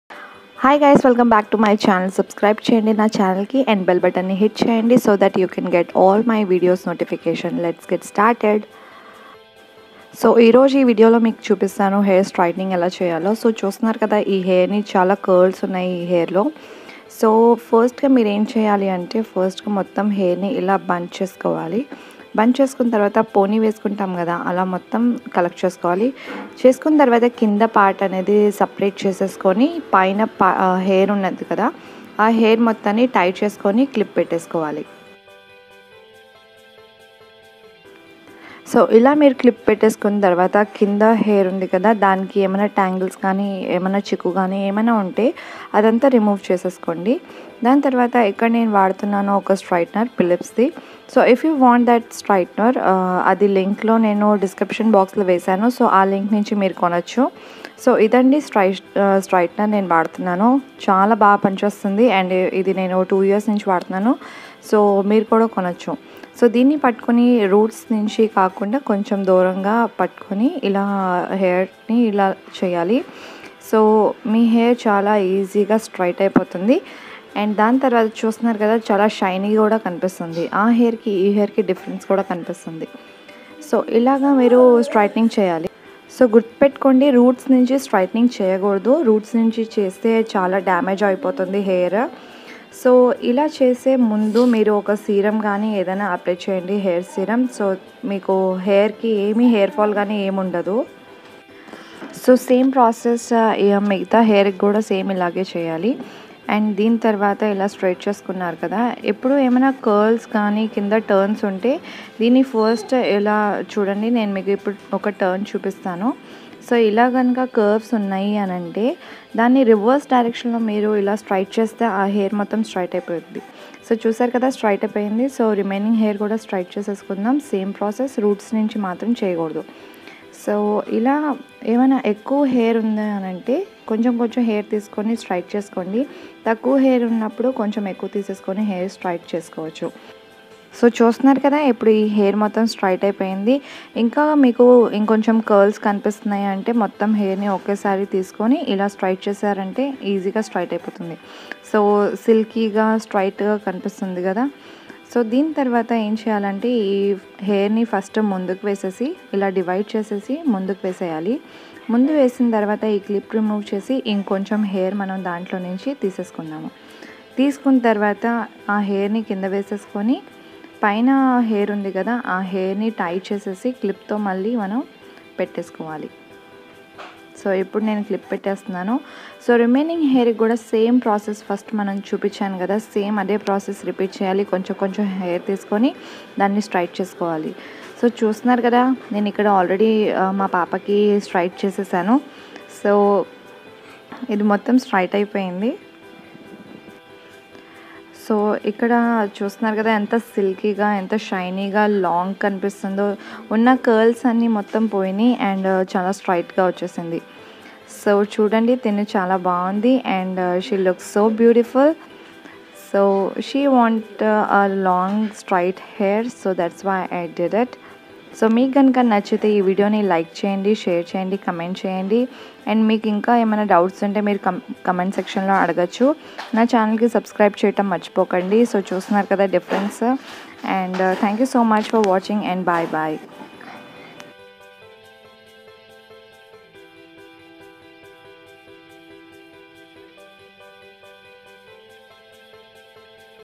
In Telugu | How To Do Hair Straightening At Home | Homemaker sravanthi
if you want to buy the hair straighter just click the link and get it